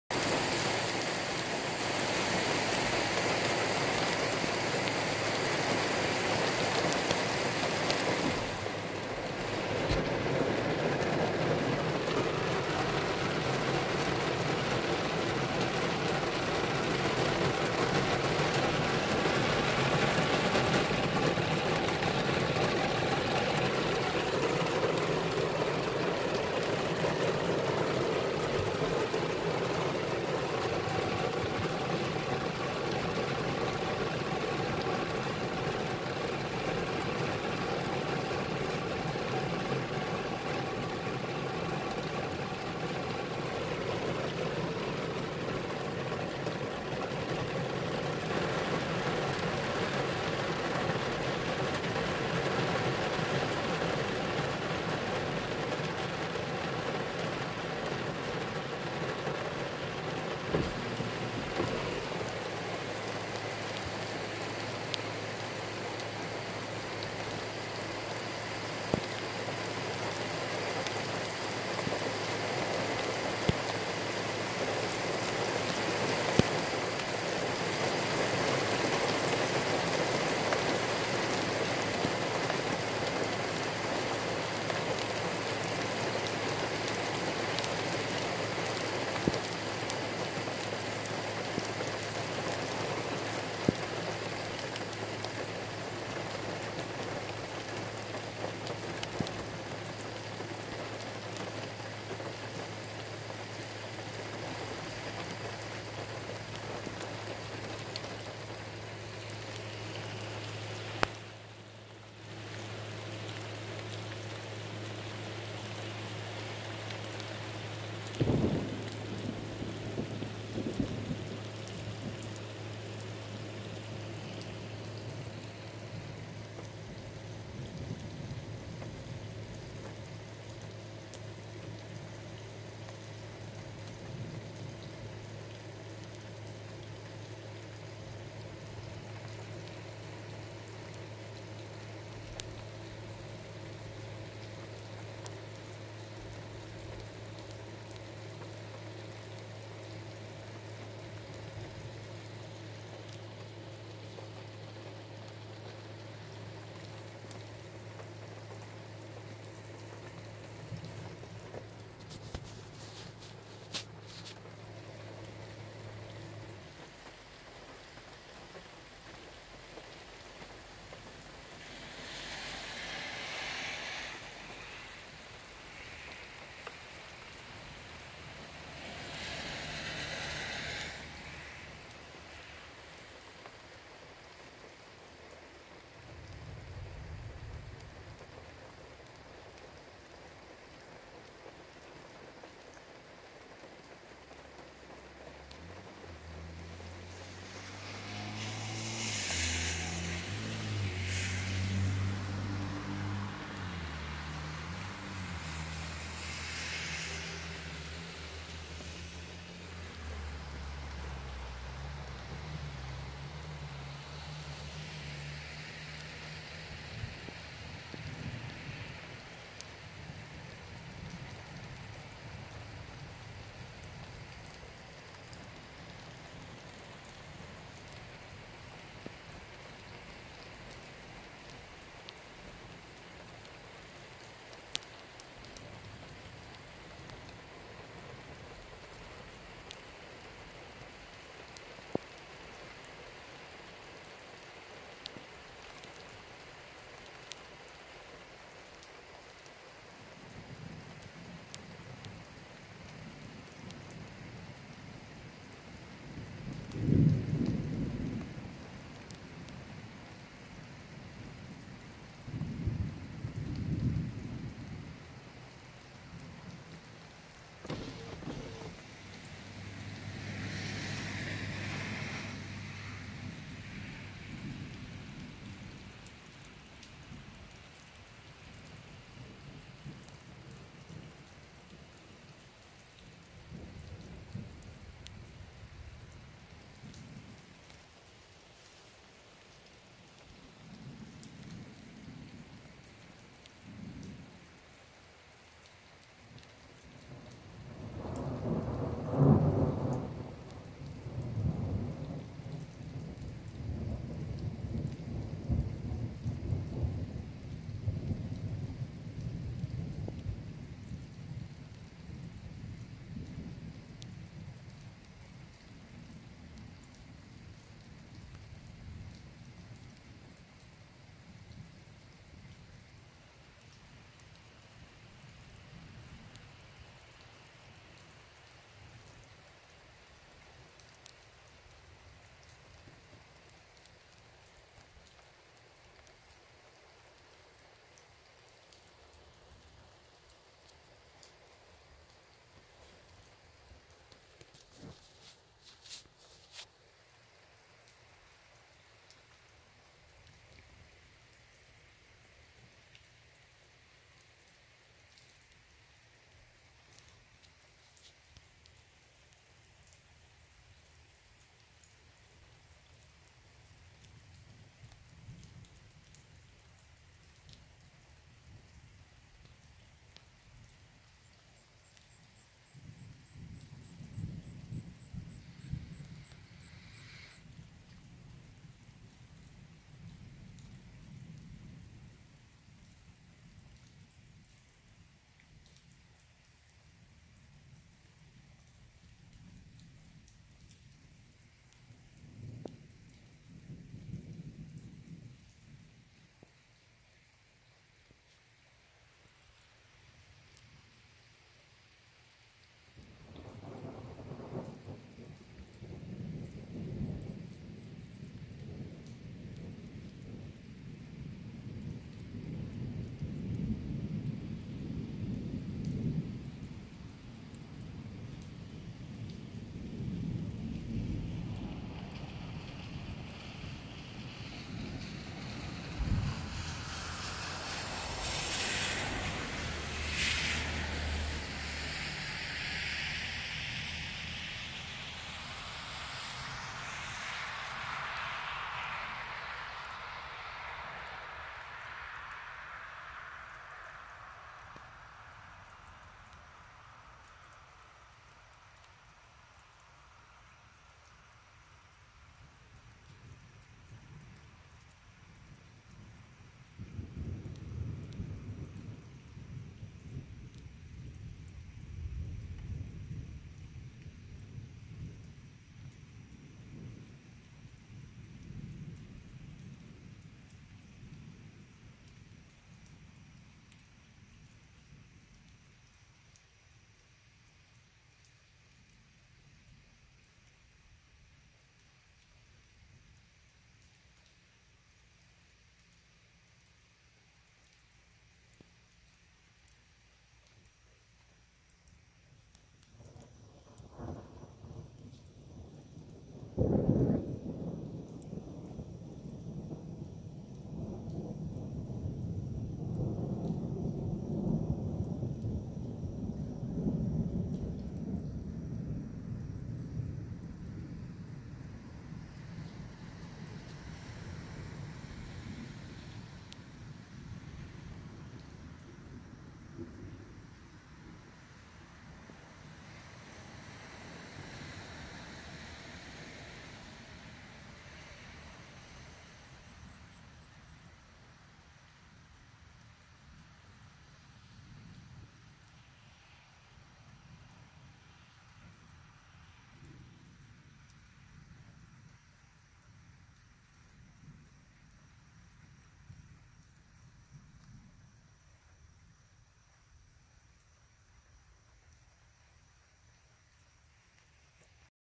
July 28th 2018 Thunderstorm in Skinquarter Va.
In between two separate storms one in Powhatan and another in Nottoway.